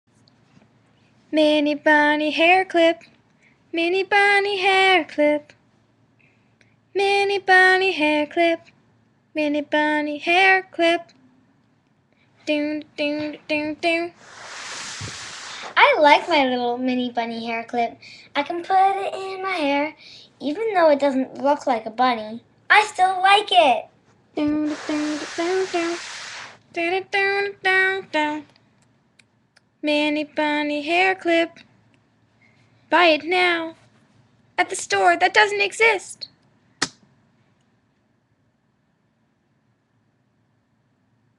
Mini Bunny Hair Clip
My little sister and I made this little "commercial" for a towel bunny that we made into a hair clip.